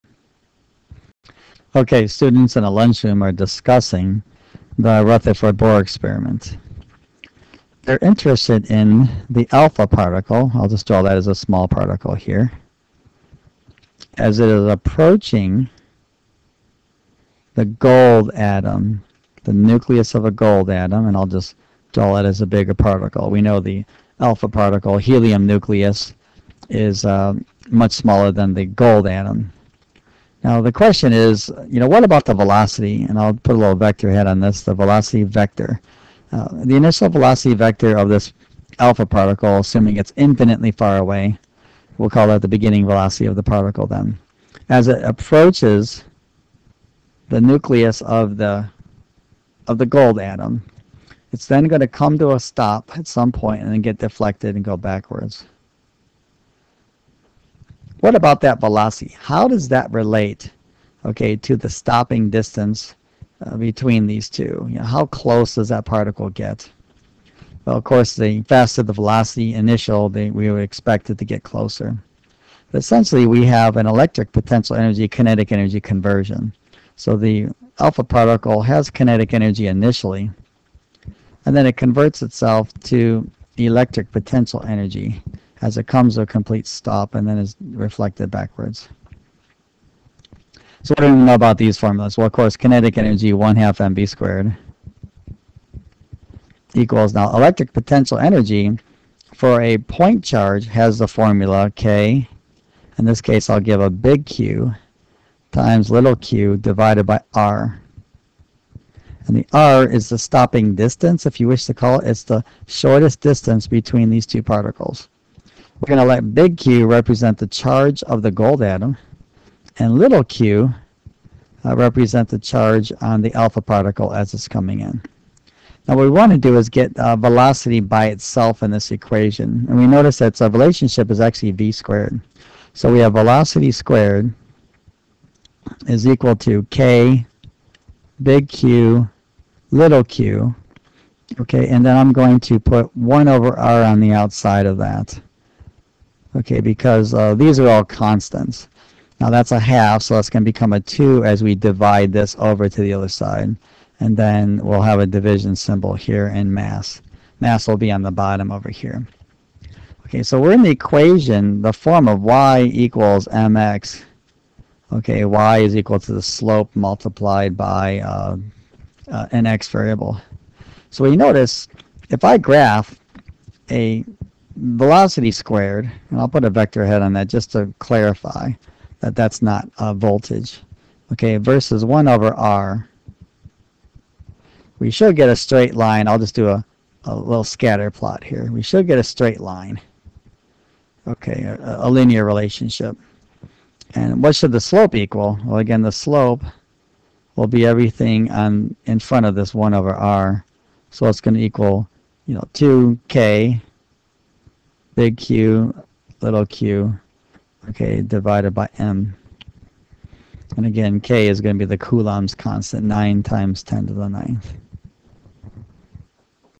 The Rutherford Alpha Particle and Stopping Distance
The relationship between the beginning velocity of an alpha particle (located at infinity) and the closest distance it gets to the nucleus of a gold atom is discussed in conjunction with the Rutherford Gold Foil Experiment.  A graph of velocity squared vs. the reciprocal of the closest distance yields a straight line.